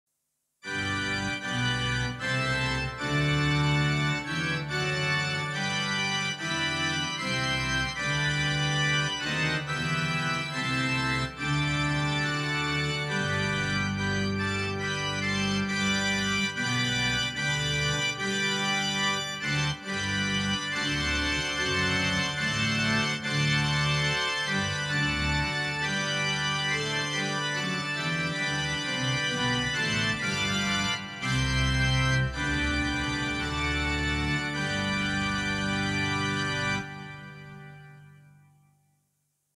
Yamaha, Modx 6, create Sound, Grand Pipe Organ, Große Kirchenorgel
St. Paul AF1&2, Volume 103, Note -12
St. Paul  AF1&2, Volume 72, Note  0
Breath Pipe, Volume 127, Note +12
Ready Pipe,  Volume 78, Note 0
Ready Pipe,  Volume 90 , Note +12